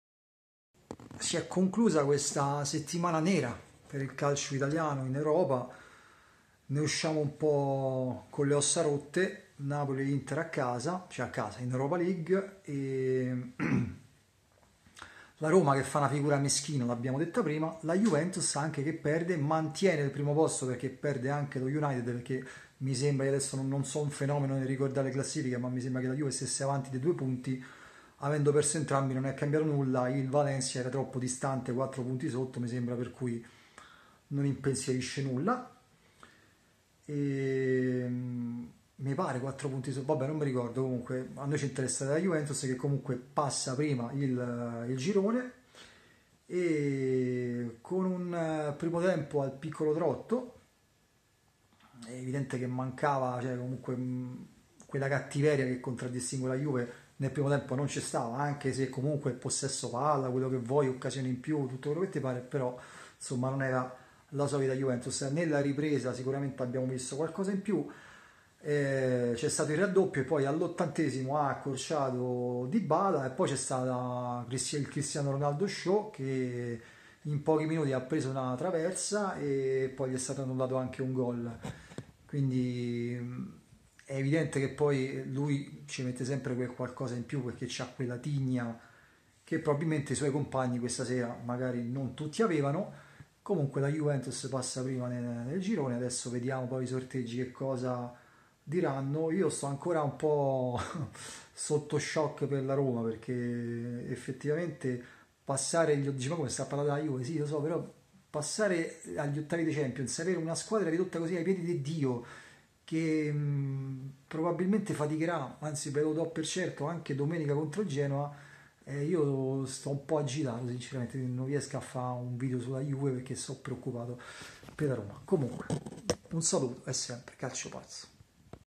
JUVE PERDE CON YOUNG BOYS MA PASSA COME PRIMA
Juventus sconfitta dallo YoungBoys grazie alla doppietta di Hoarau che rende inutile il gol di Dybala nel finale. La Juventus gestisce il possesso palla ma nonostante le occasioni create va in svantaggio per il gol dal dischetto di Hoarau alla mezz'ora. Nella ripresa squadra di Allegri più intraprendente ma soltanto dopo il raddoppio di Hoarau in contropiede e l'ingresso di Dybala la Juventus chiude gli svizzeri nella propria area, trova il gol con l'argentino e nel finale un gol annullato e altre occasioni sprecate non permettono ad Allegri di tornare in italia con un punto.